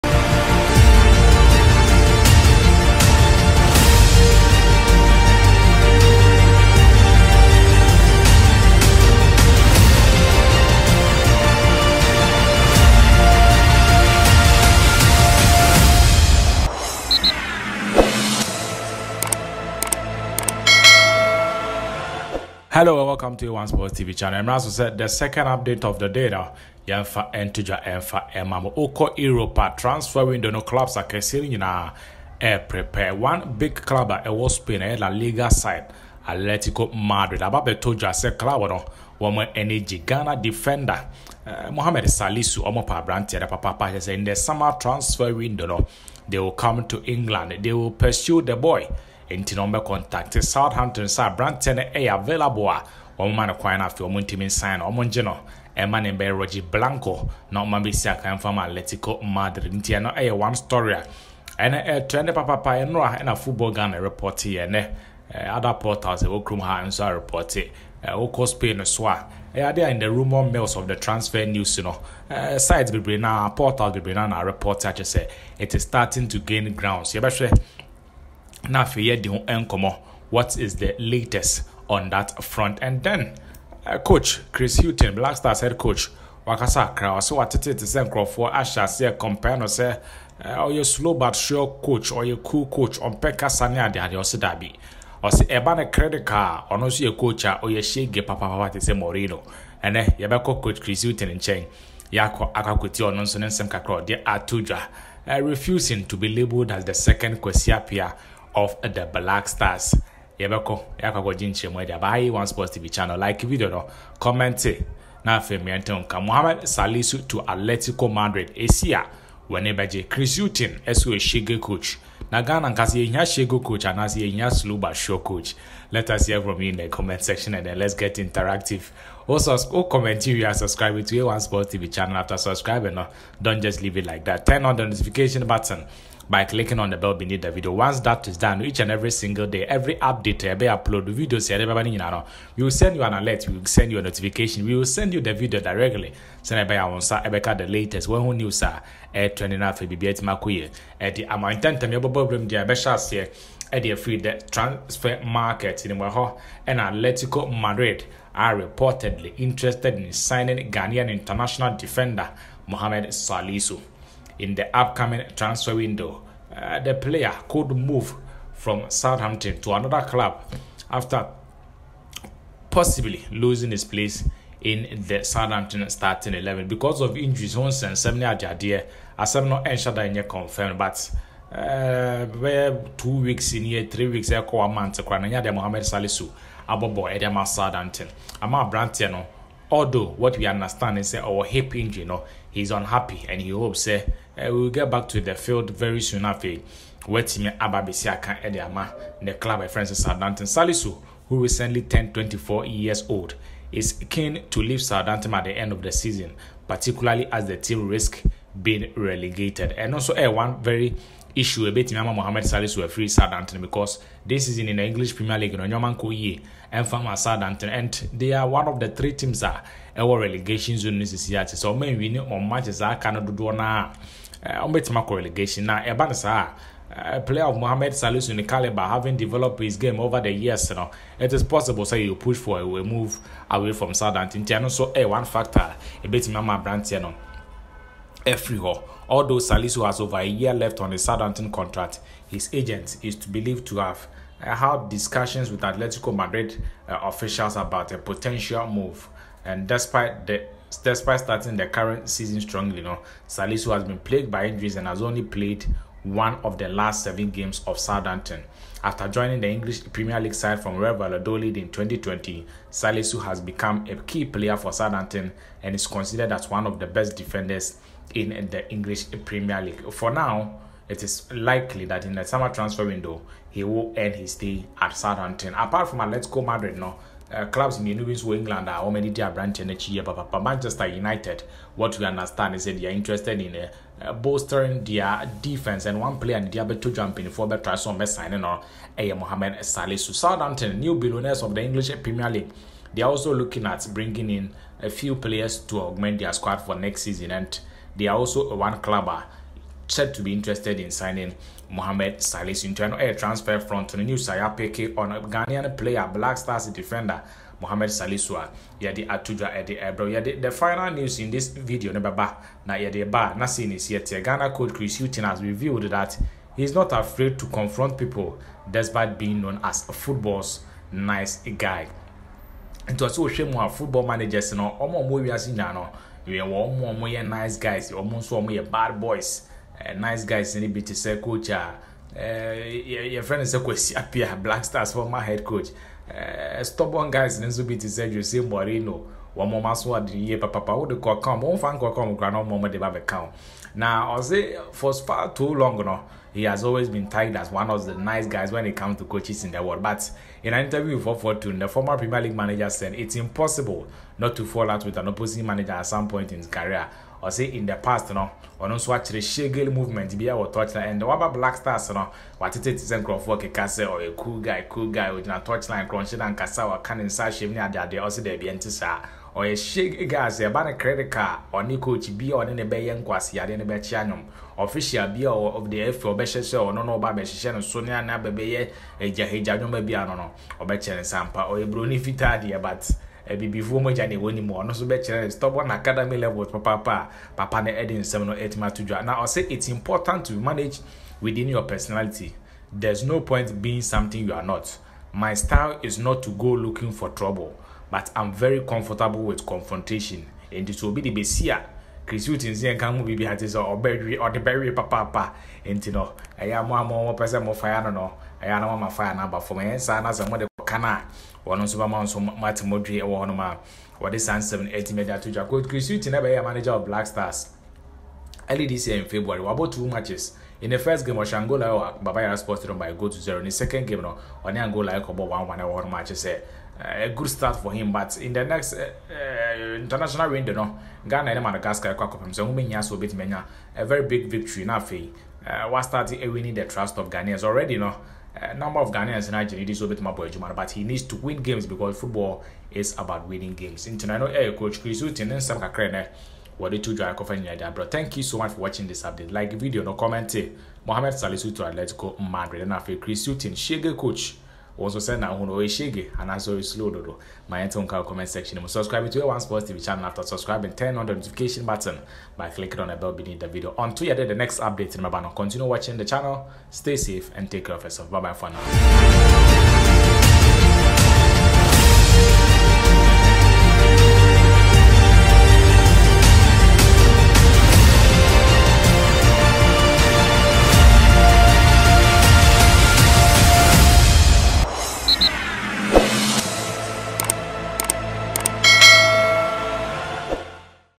ATL. MADRID WANTS TO SIGN MOHAMMED SALISU | CHRIS HUGHTON REVEALS ‘BAD BOY’ SIDE
Ghana defender, Mohammed Salisu has been linked with a move to Atletico Madrid from Southampton.
The centre-back is also wanted by English Premier League giants Manchester United and revived Newcastle United.
Meanwhile, Black Stars head coach Chris Hughton has indicated that he has a bad boy side and is not afraid of confrontation.